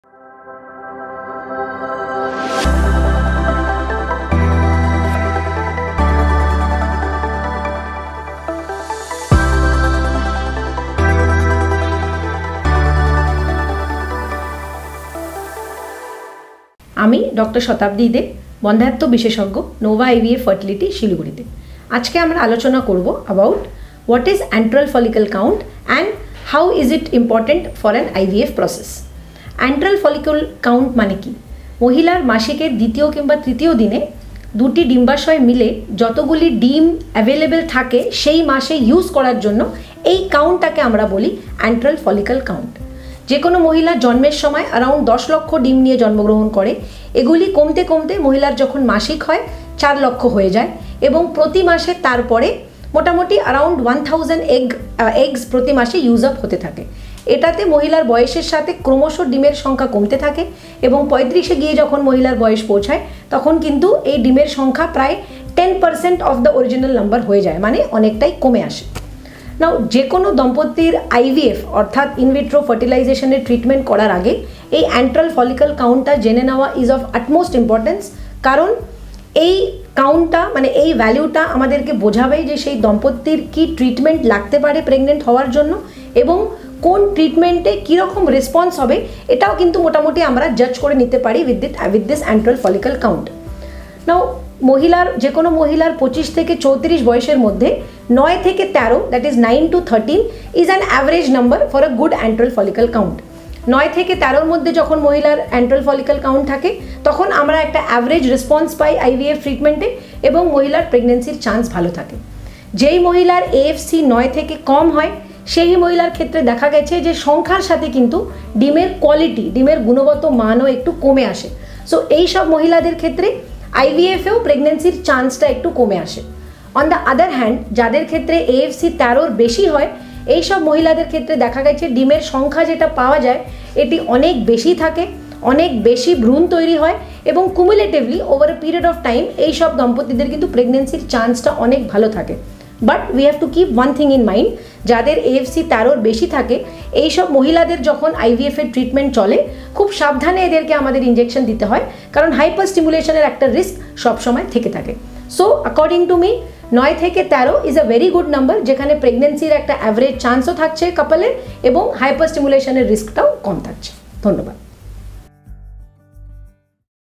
What is Antral Follicle Count? | Dr Satabdi Dey | Fertility Specialist | Nova IVF- Siliguri
Dr. Satabdi Dey, Fertility Specialist at Nova IVF- Siliguri talks about-
What is Antral Follicle Count?

The number of eggs present in the two ovaries to be used in that month on the second or third day of a woman's menstrual cycle is known as the antral follicle count. When a woman gives birth, she has about 10 lakh eggs in reserve; by the time she receives her first meal, that number has dropped to 4 lakh. And with each successive monthly cycle, almost a thousand eggs are consumed.

As a result, the egg count rapidly declines with age, and by the time she is 35, she only has access to 10% of her initial egg count. Therefore, it is crucial to understand a couple's antral follicle count before beginning IVF treatment. Because of this count, we will be able to determine the sort of therapies the couple will require and determine which form of treatment will produce what kind of reaction.

Therefore, 9 to 13 is an average range for a healthy antral follicle count in women between the ages of 25 and 34.We receive an average response to our IVF treatment and have decent pregnancy odds when the antral follicle count is between 9 and 13.

 So the chances of pregnancy in such women decrease. On the other hand, when the count is more than 13 in such women, we have seen that the Egg count is higher, which means there are more embryos, and cumulatively, such couples have a greater chance of pregnancy.

One thing, however, must be kept in mind: Since there is a chance of hyperstimulation with such women who have an AFC of greater than 13, injections must be administered with caution. Therefore, I believe that 9 to 13 is a very nice range where the couple still has a decent probability of becoming pregnant while also having a lower risk of hyperstimulation.

0:00 Introduction
0:10 What is Antral Follicle Count?
0:29 second or third day of a woman's menstrual cycle
0:56 egg count decreases
1:10 before proceeding for IVF
1:43  chances of pregnancy
1:53 quality gets diminished
2:22 risk of hyperstimulation